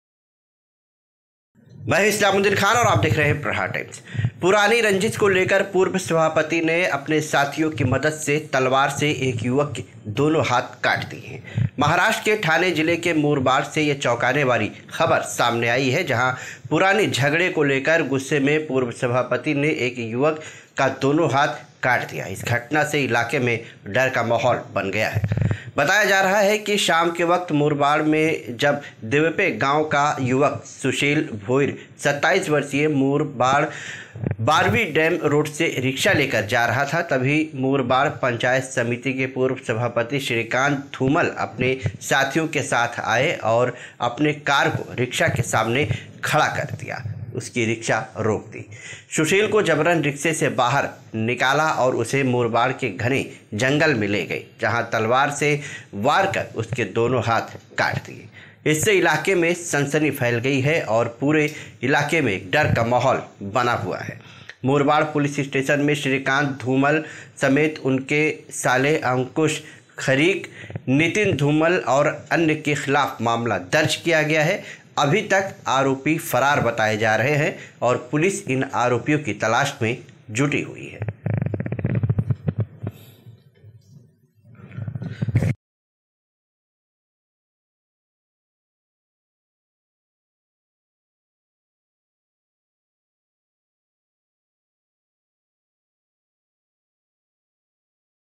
पहले रास्ता रोका फिर, बेराहमी से दोनो हाथ युवक के का-ट दिया || Islamuddin Khan || Aapka Prahar Times
पहले रास्ता रोक फिर बेराहमी से दोनो हाथ युवक के का-ट दिया || Islamuddin Khan

पुरानी रंजिश को लेकर पूर्व सभापति ने अपने साथियों की मदद से तलवार से युवक के दोनों हाथ का-ट दिए,, महाराष्ट्र के ठाणे जिले के मुरबाड से चौंकाने वाली खबर आई है. जहां पुराने झगड़े को लेकर गुस्से में पूर्व सभापति ने एक युवक का दोनो हाथ का-ट दिया, इस घटना से इलाके में डर का माहौल फैल गया है.बताया जा रहा है कि शाम के वक्त मुरबाड में जब देवपे गांव का युवक सुशील भोईर (27) मुरबाड बारवी डैम रोड पर रिक्शा से जा रहा था, तभी मुरबाड पंचायत समिति के पूर्व सभापति श्रीकांत धूमल अपने साथियों के साथ आए और अपने कार को रिक्शा के सामने खड़ी करके रिक्शा को रोका. सुशील को जबरन रिक्शा से बाहर निकाला और उसे मुरबाड के घने जंगल मे ले गए. जहां तलवार से वार कर उसके दोनों हाथ काट दिए. इससे इलाके में सनसनी फैल गई है और पूरे इलाके में डर का माहौल फैल गया है. मुरबाड पुलिस स्टेशन में श्रीकांत धूमल समेत उनके साले अंकुश खरीक, नितिन धूमल और अन्य के खिलाफ मामला दर्ज किया गया है. अभी तक आरोपी फरार बताए जा रहे है पुलिस आरोपियों की तलाश कर रही हैं.